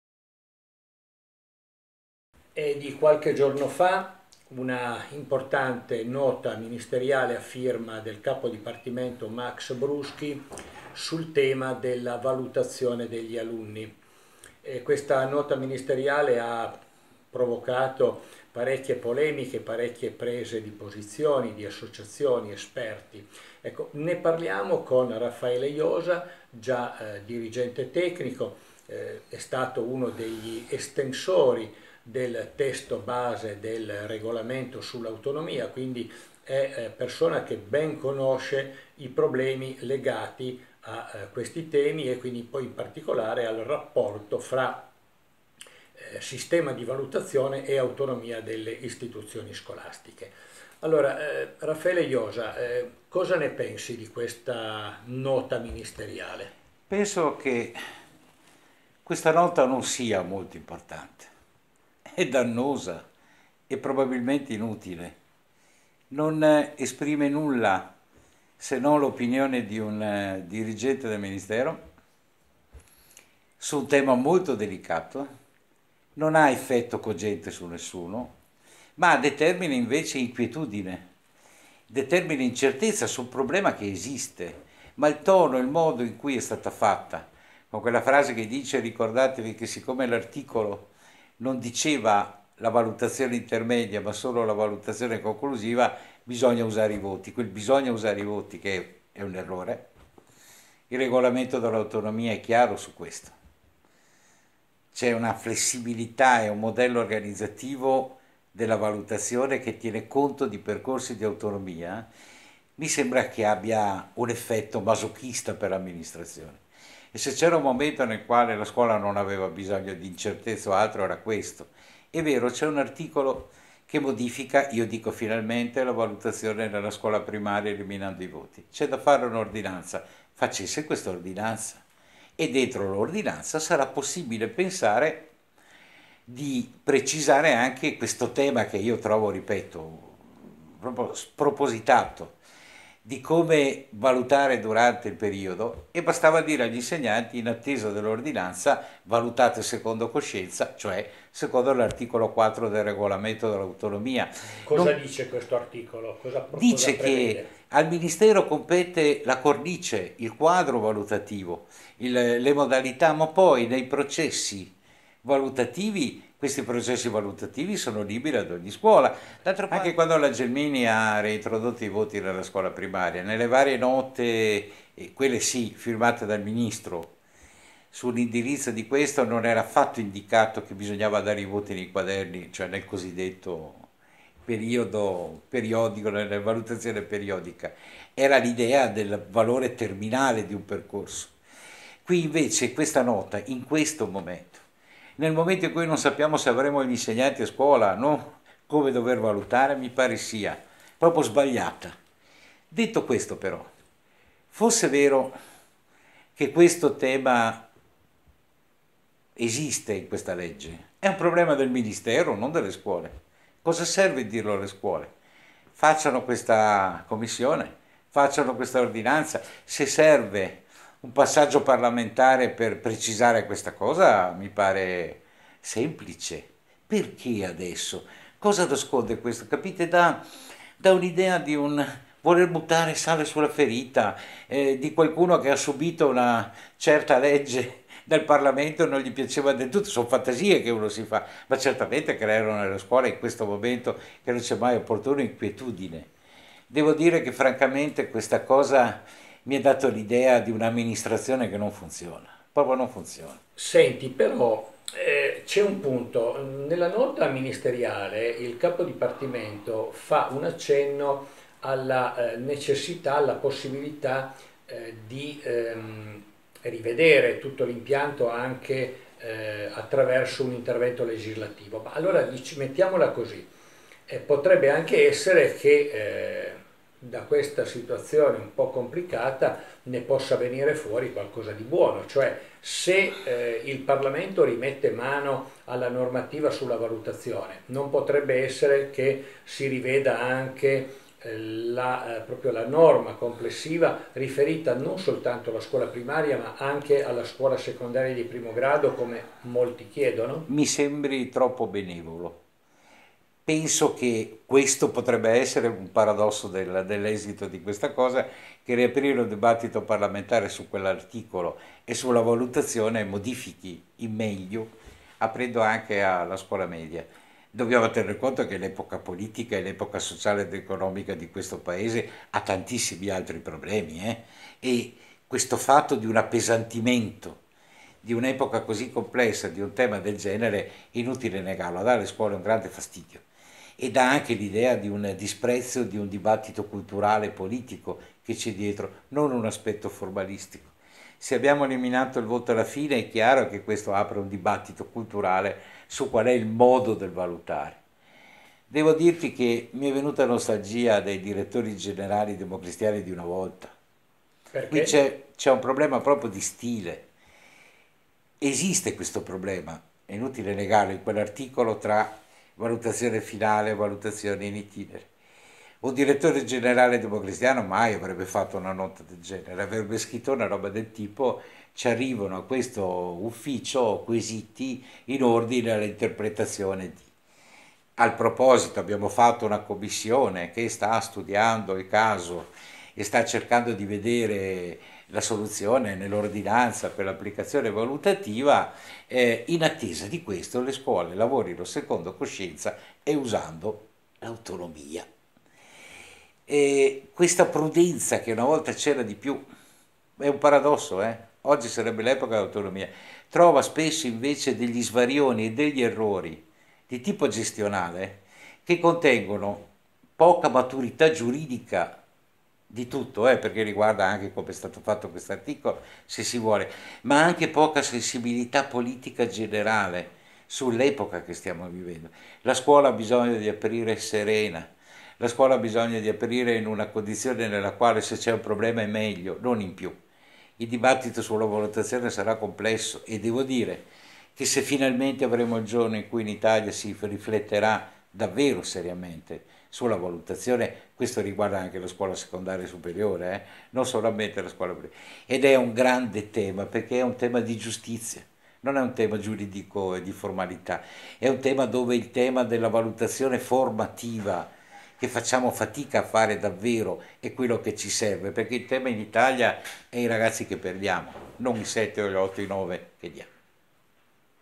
Valutazione alla primaria. Iosa: La nota del ministero è inutile
Raffaele Iosa, pedagogista, è stato per molti anni dirigente tecnico del Ministero dell'Istruzione. E' uno degli estensori del testo del Regolamento
dell'autonomia ed è stato anche responsabile dell'Osservatorio nazionale
sull'handicap.
Intervista a cura di Reginaldo Palermo